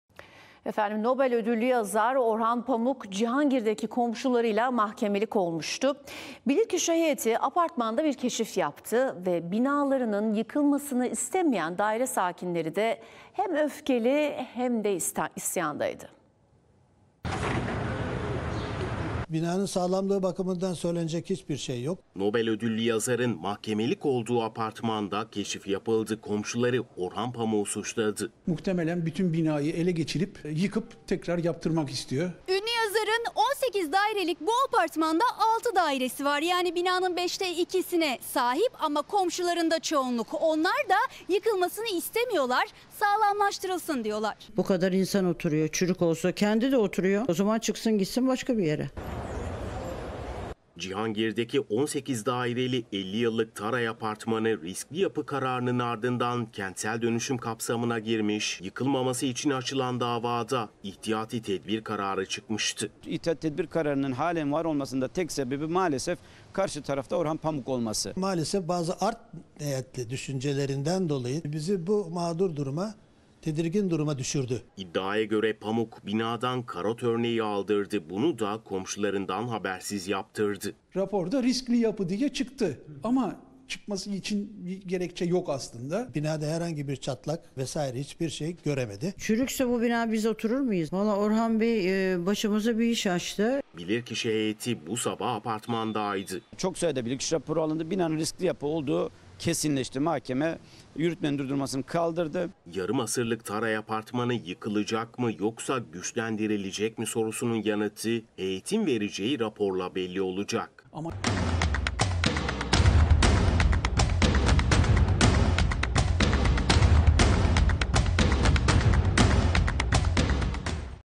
Orhan Pamuk'un apartmanında keşif!
NOBEL ÖDÜLLÜ YAZAR ORHAN PAMUK CİHANGİR'DEKİ KOMŞULARIYLA MAHKEMELİK OLMUŞTU... BİLİRKİŞİ HEYETİ APARTMANDA KEŞİF YAPTI...BİNALARININ YIKILMASINI İSTEMEYEN DAİRE SAKİNLERİ İSE HEM ÖFKELİ,,, HEM DE İSYANDAYDI.